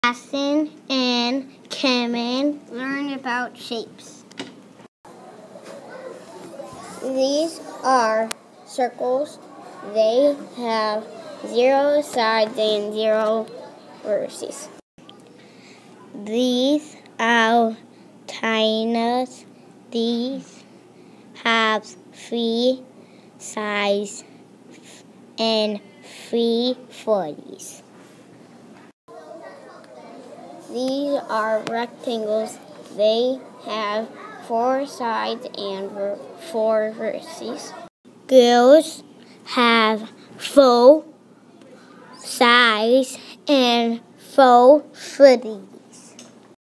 Jackson and Cameron Learn About Shapes
MTH.01.01: understands and uses content-specific vocabulary

MTH.01.06: partitions, composes and decomposes shapes